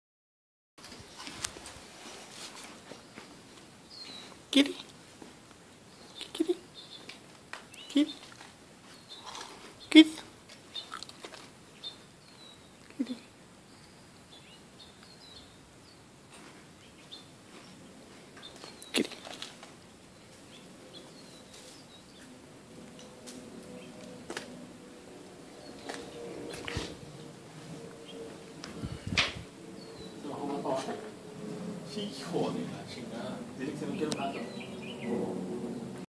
Venado en el trabajo
Casi se deja tocar